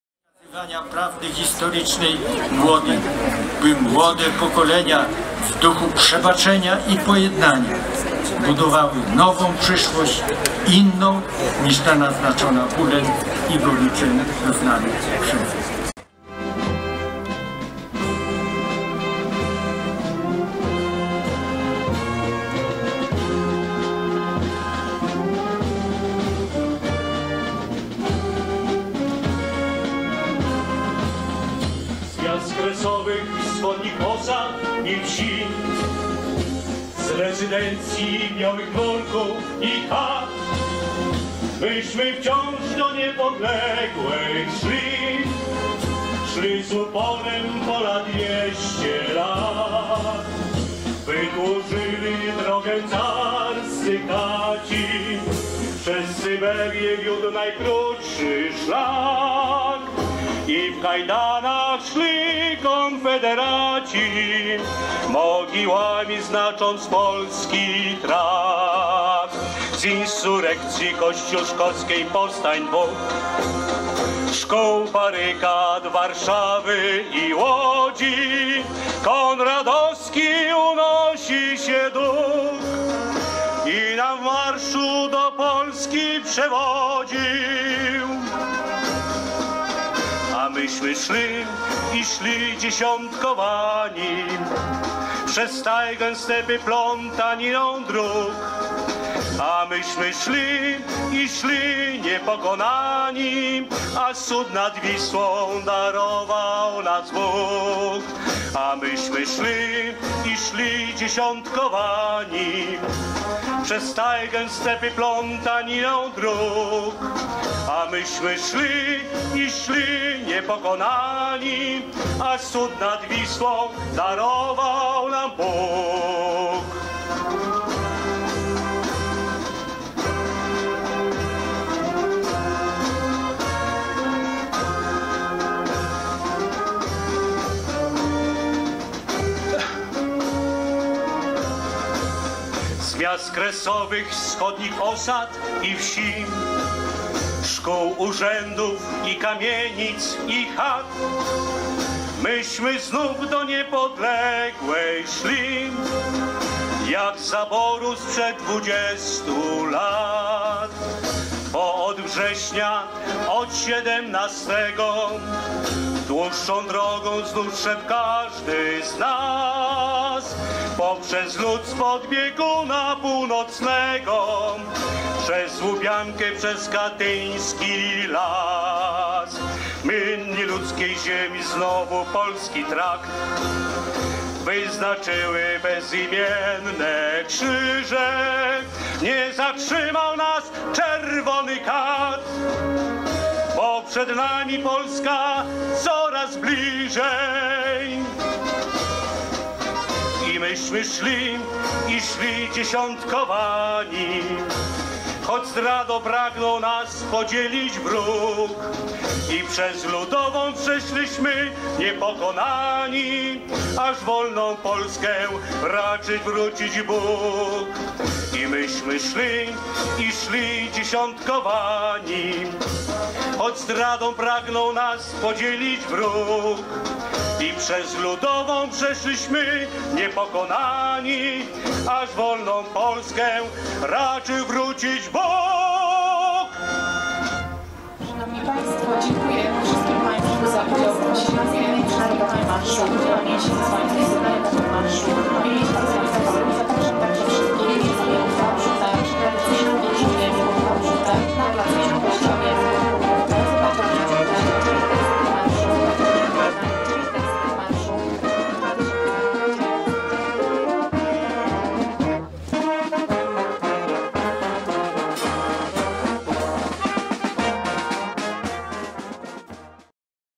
Międzynarodowy Marsz Pamięci Zesłańców Sybiru - Białystok 2018
W związku ze stuleciem odzyskania przez Polskę niepodległości tegoroczny Międzynarodowy Marsz Pamięci Zesłańców Sybiru odbył się pod hasłem "Przez Sybir do niepodległej". Przeszedł ulicami Białegostoku już po raz osiemnasty.
Białystok - 7 wrzesień 2018 r.